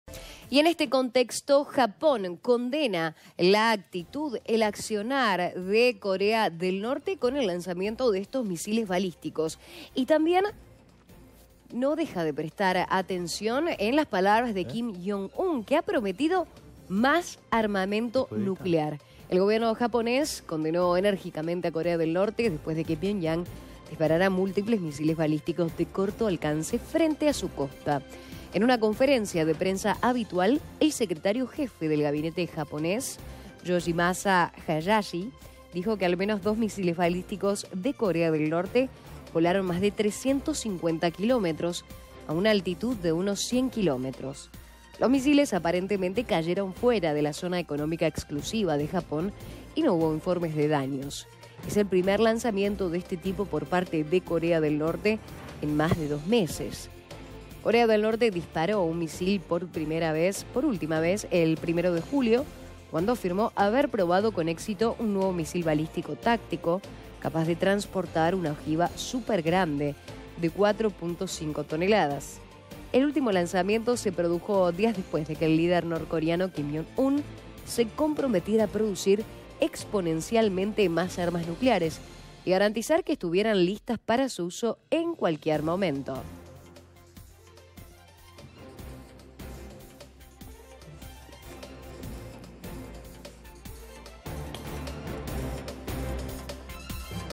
Japón rechaza el reciente lanzamiento de misiles por parte de Corea del Norte
👉 El gobierno japonés condenó enérgicamente a Corea del Norte después de que Pyongyang disparara múltiples misiles balísticos de corto alcance frente a su costa.